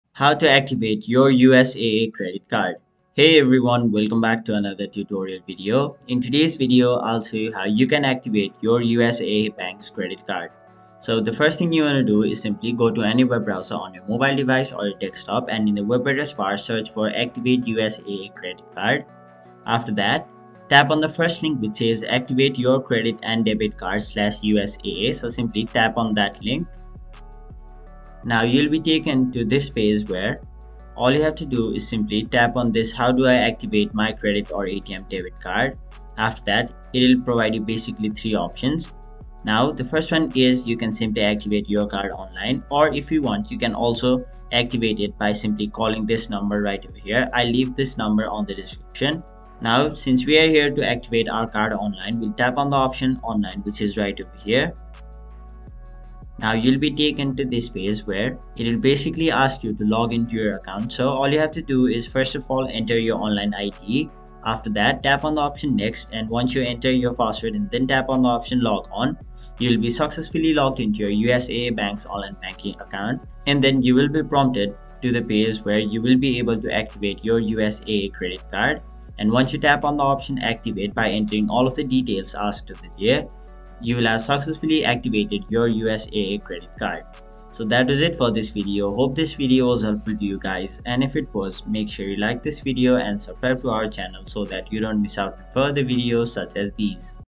USAA Credit Card - Activate Online | 2023
In this video, I will guide you on How to Activate USAA Bank Credit Card.